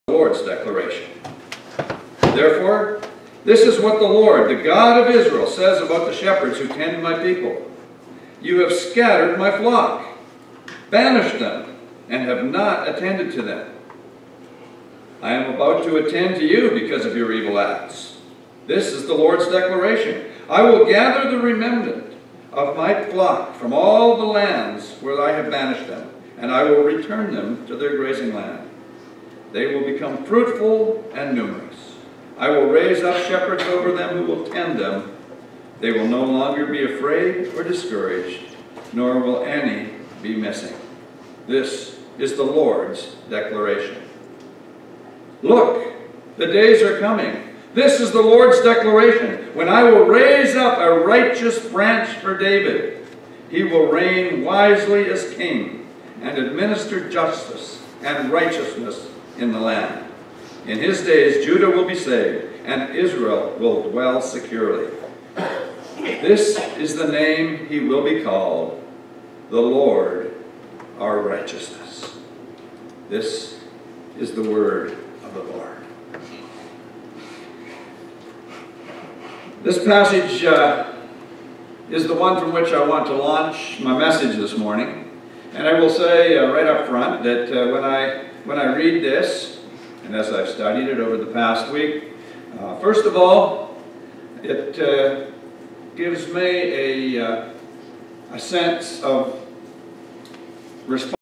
The Faster Pastor, Delisle Community Chapel, Pastor Herb Anderson
Pastor Herb preaches a message every Sunday at Delisle Community Chapel. These videos are posted on our